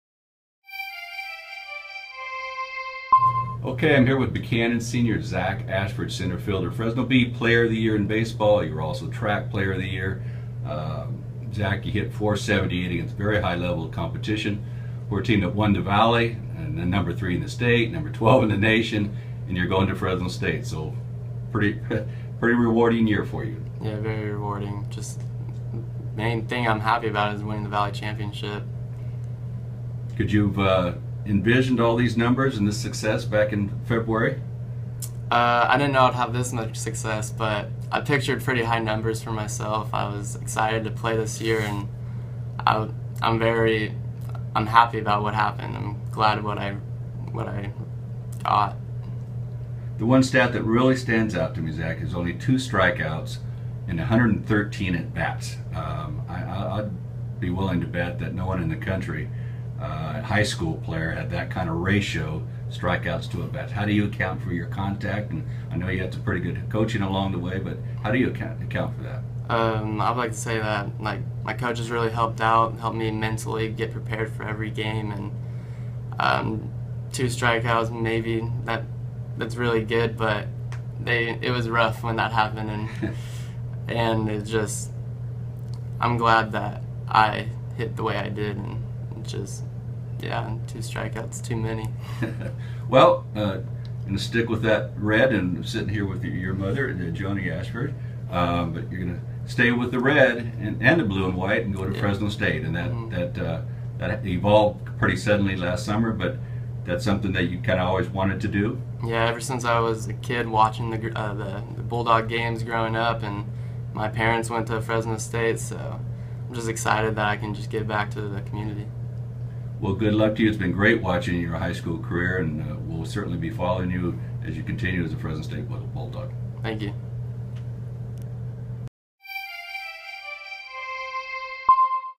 Fresno Bee Player Of The Year In Baseball Is About An Extraordinary Zach Attach
Buchanan senior Zach Ashford hits .478 and strikes out but two times in 113 at bats.